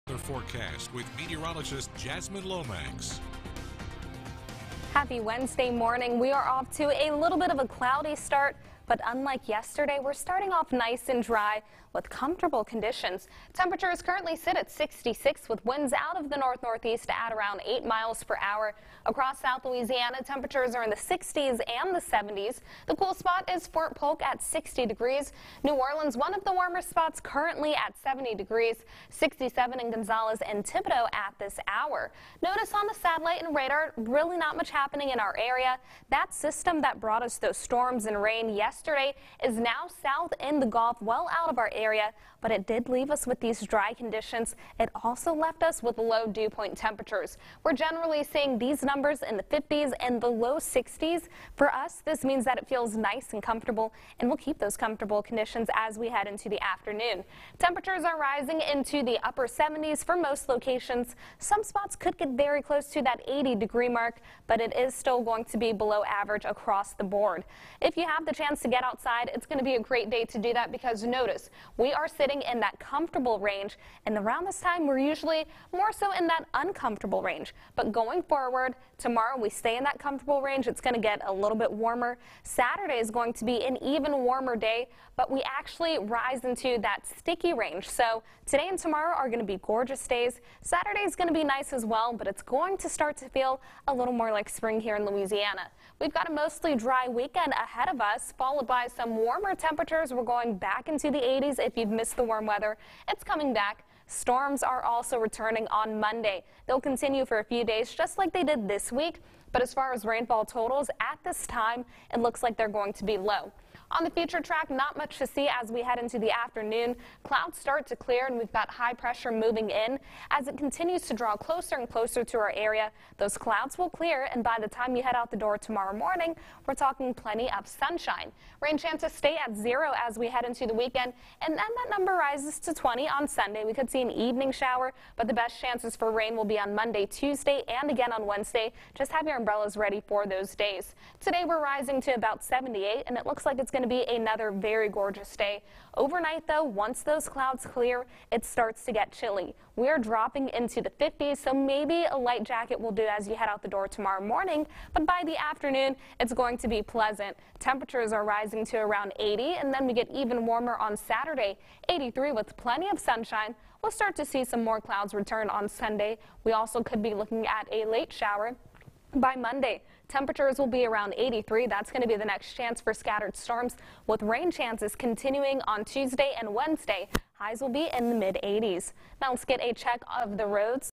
StormTracker Forecast (May 13, 2021)
The dry weather is here, but showers and storms will return next week.